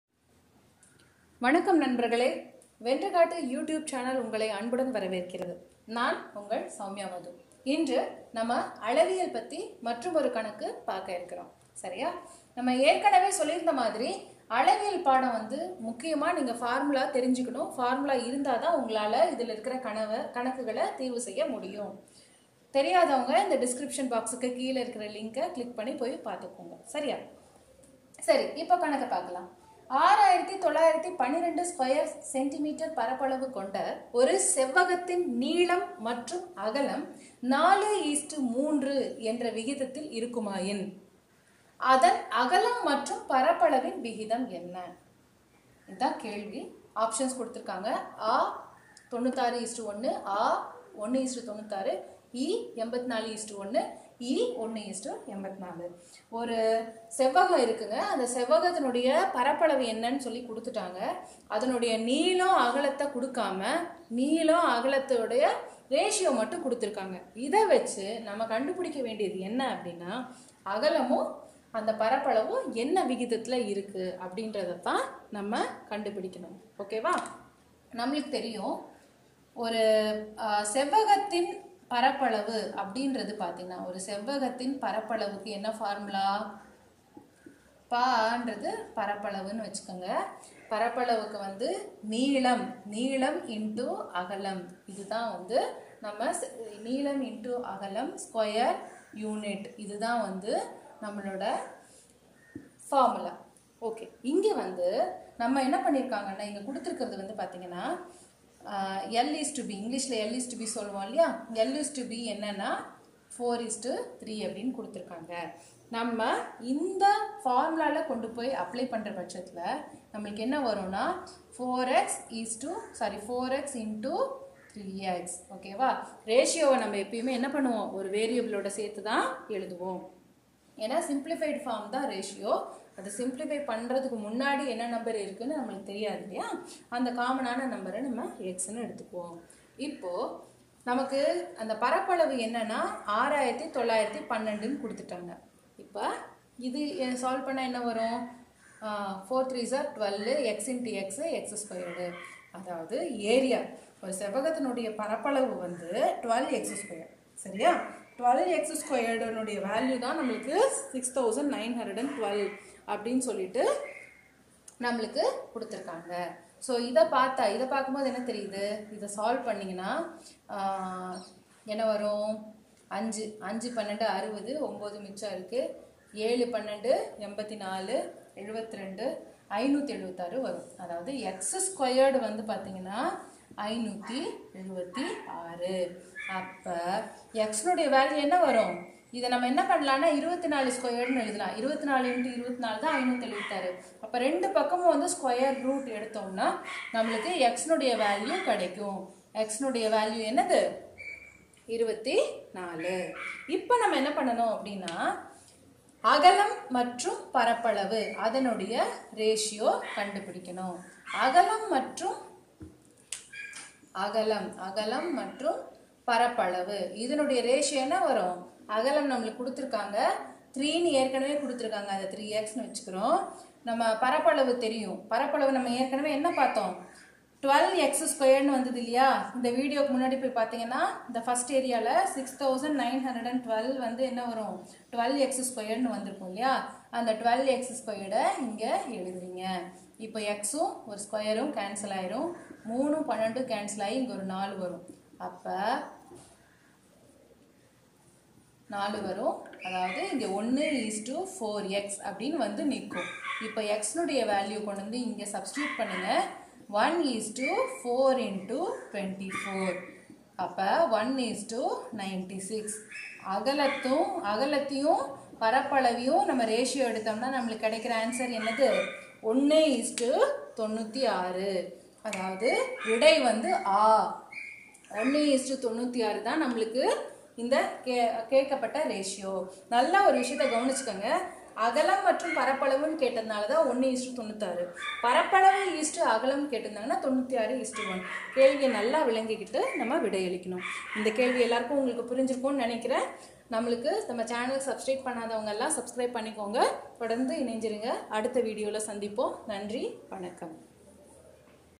6912 சதுர செ.மீ பரப்பளவு கொண்ட ஒரு செவ்வகத்தின் நீளம் மற்றும் அகலம் 4:3 என்ற விகித்த்தில்.......
அளவியல் பகுதி திறனறி வினாக்களில் முக்கியமான பகுதி ஆகும்.எனவே நன்றாக பயிற்சி மேற்கொள்ளுங்கள்.